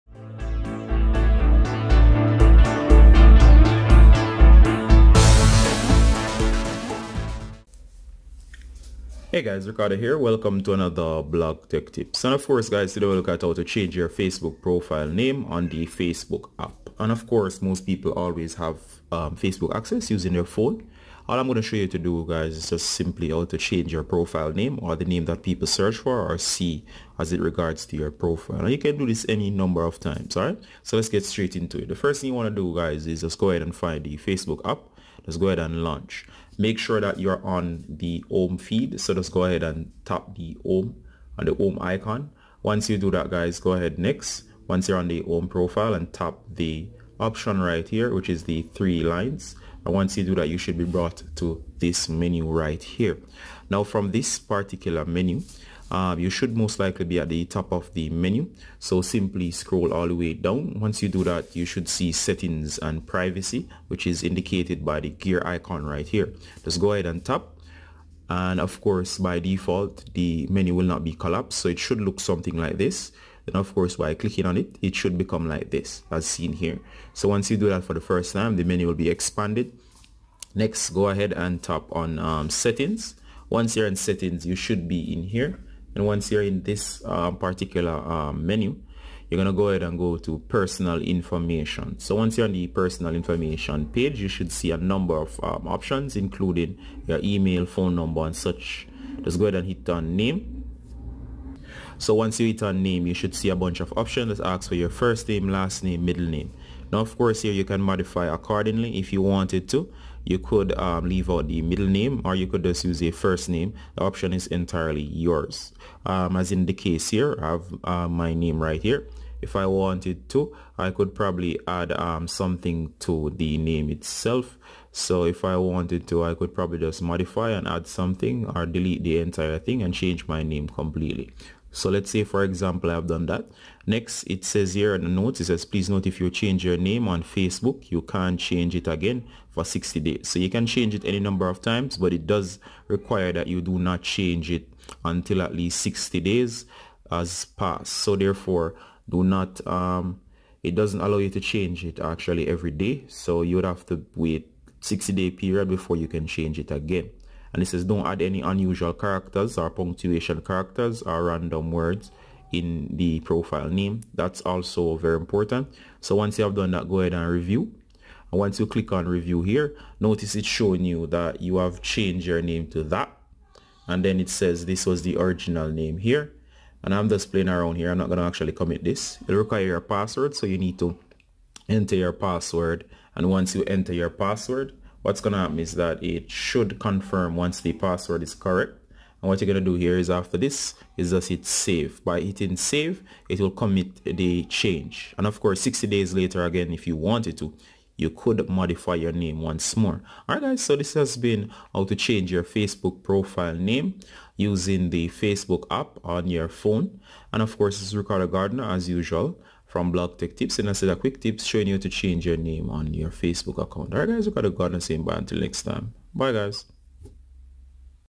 Change your Facebook Profile Name in Facebook App
This is how to Change your Facebook Profile Name in Facebook App. Change your profile name to whatever you want right now easily as shown in this tutorial.